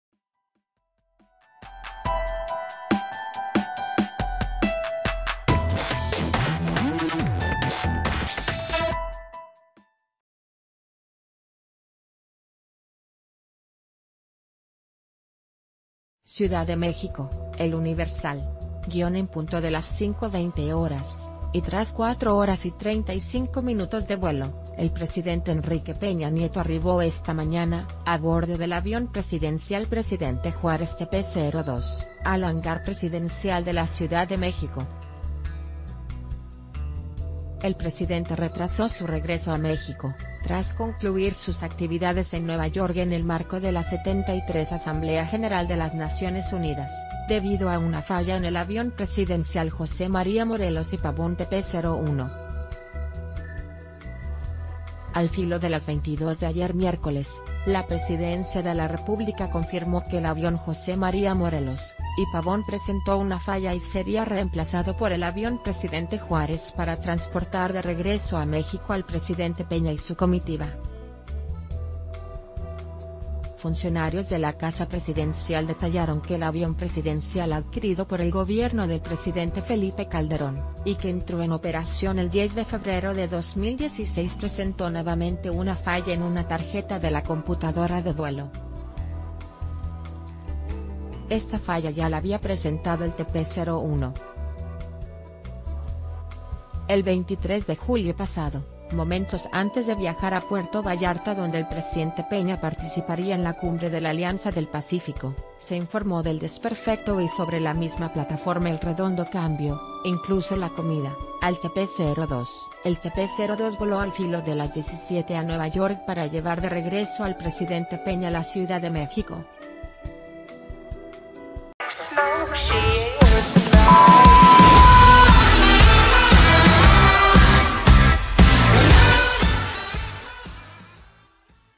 Peña Nieto llega a la CDMX tras falla en avión presidencial - El Diario de Yucatán
CIUDAD DE MÉXICO (El Universal).- En punto de las 5:20 horas y tras cuatro horas y 35 minutos de vuelo, el Presidente Enrique Peña Nieto arribó esta mañana, a bordo del avión presidencial “Presidente Juárez” TP-02, al Hangar Presidencial de la Ciudad de México. El Presidente retrasó su regreso a Méx...